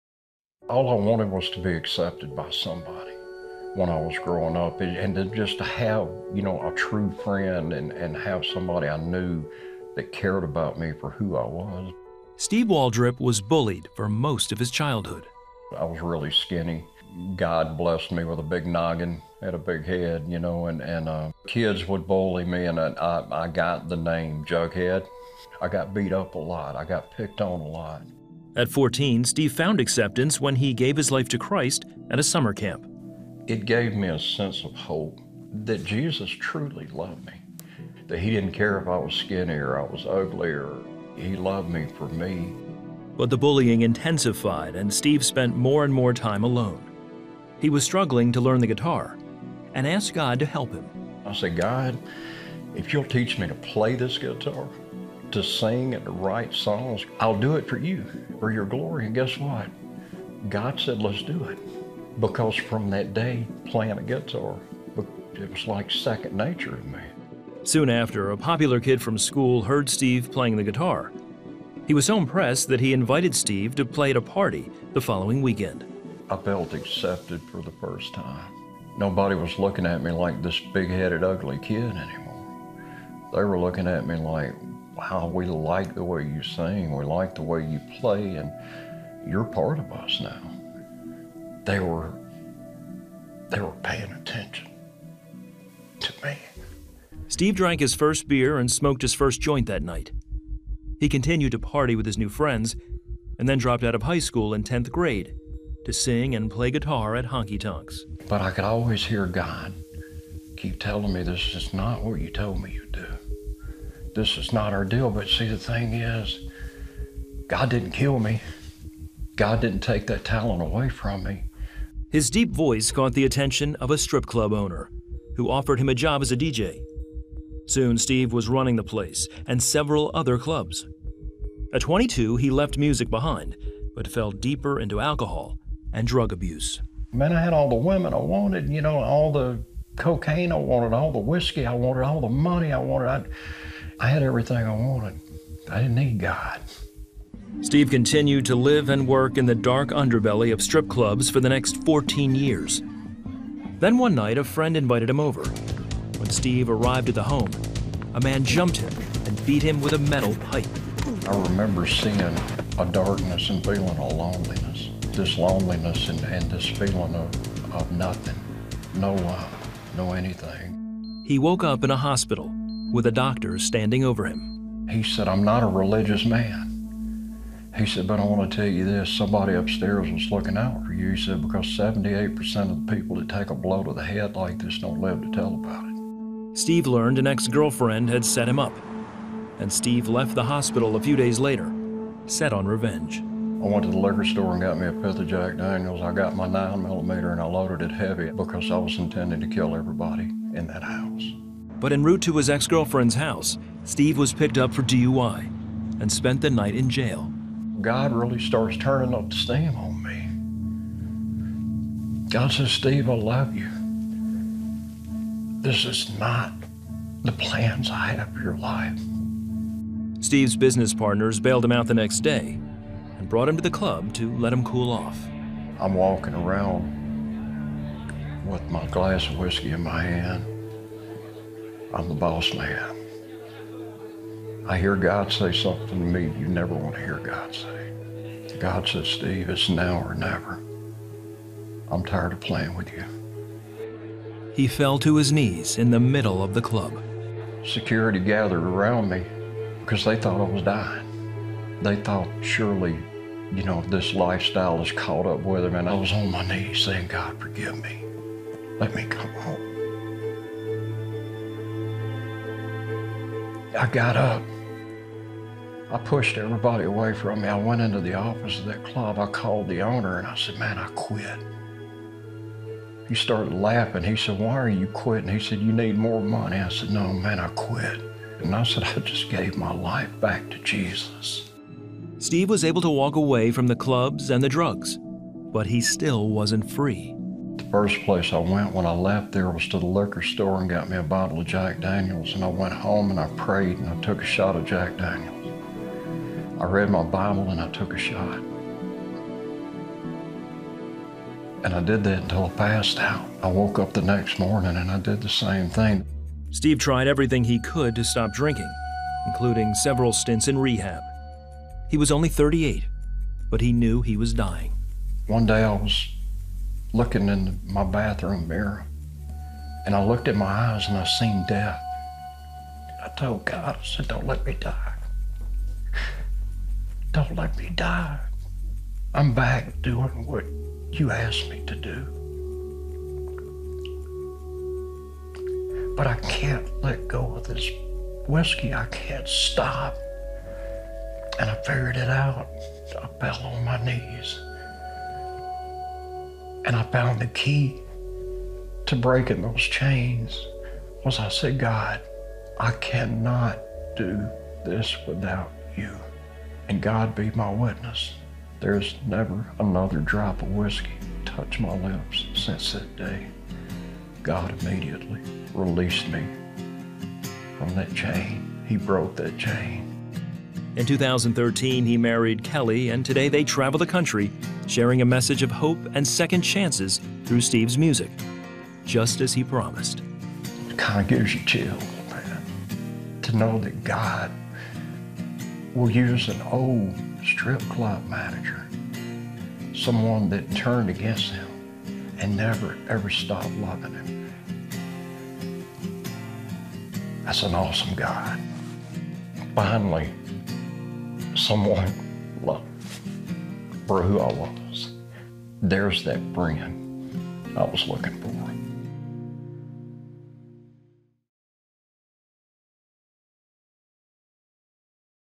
He Had Everything, But Gave it Up
Steve had all the drugs, booze, and women he could ever want, but when his lifestyle nearly cost him his own life, he realized he was out of control.